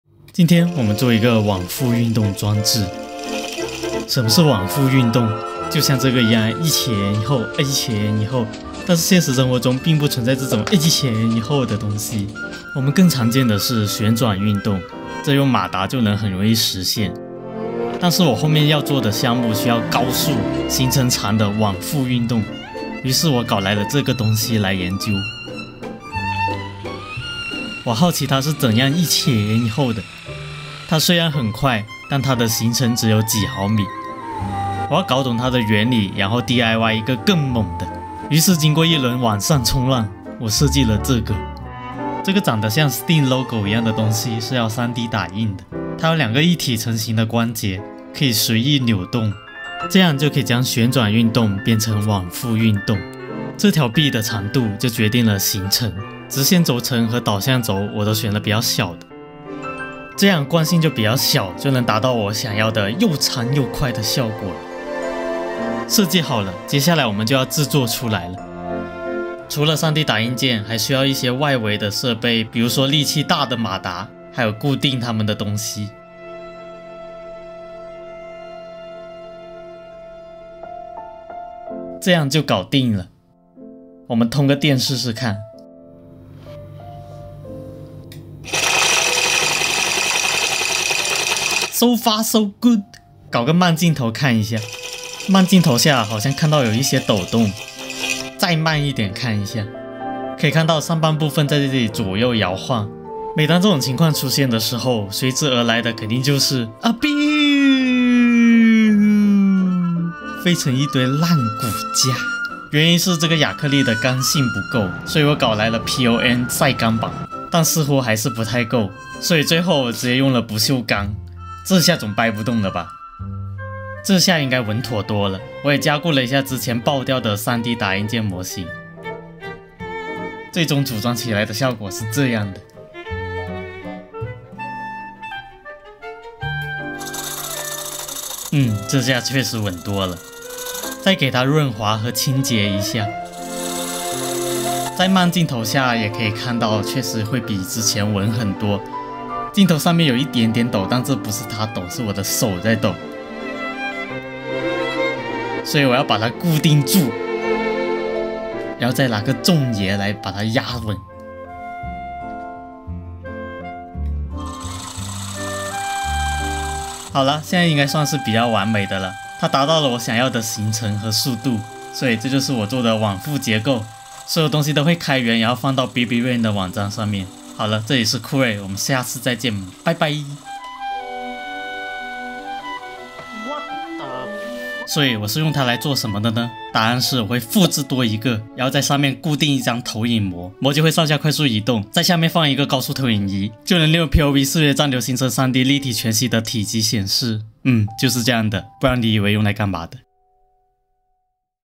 如何制作一个往复运动装置？
我的酷睿比比，Corebb。
这是我制作的一个往复运动装置

它是开源的！

你还可以进我的频道中观看我以前制作的许多有趣的东西